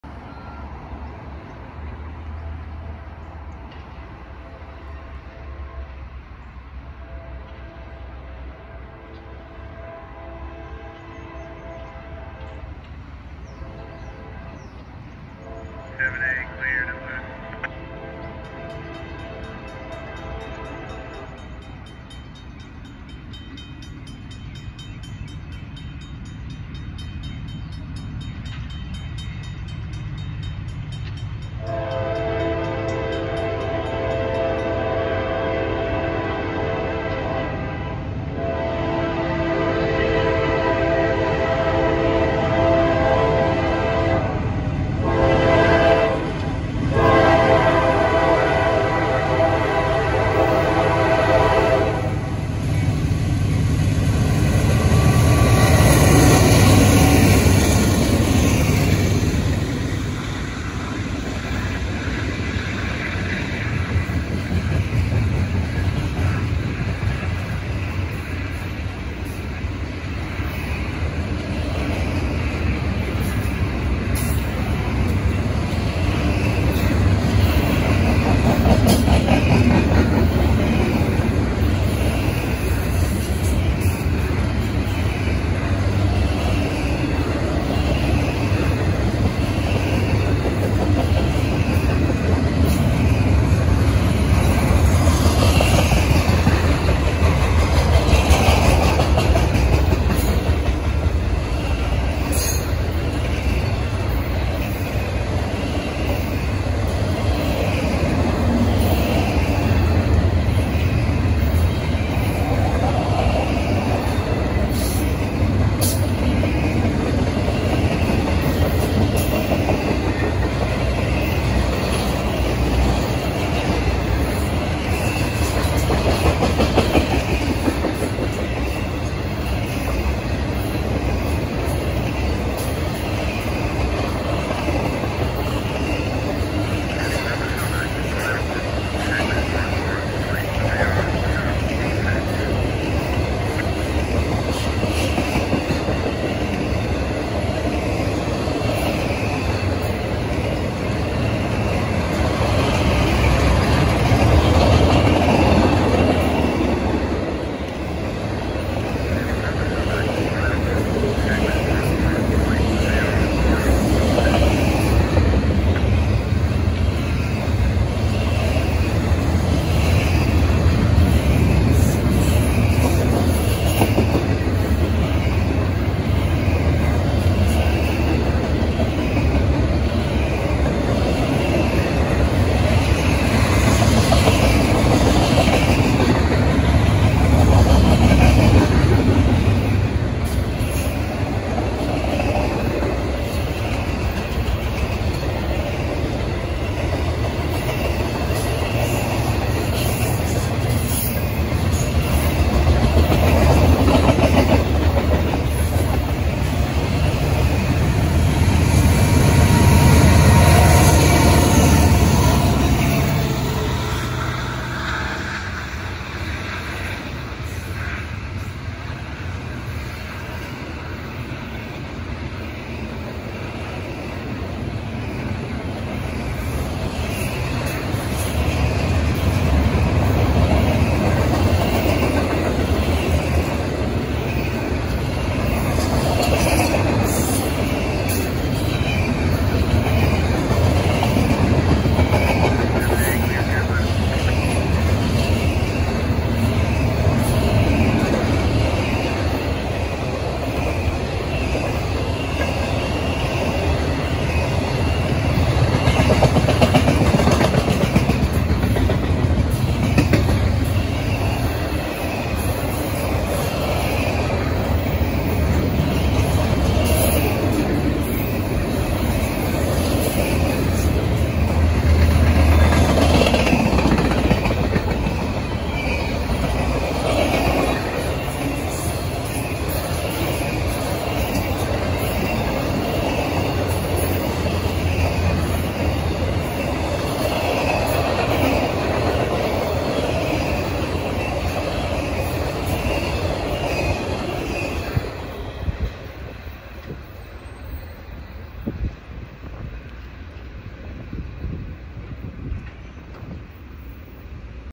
NS 4582 Leads NS 27A (With Mid-Train DPU's) Thru Duluth, GA 3/24/2023
NS 4582
CSX 7901

Mid-Train DPU's:
NS 4404
NS 1082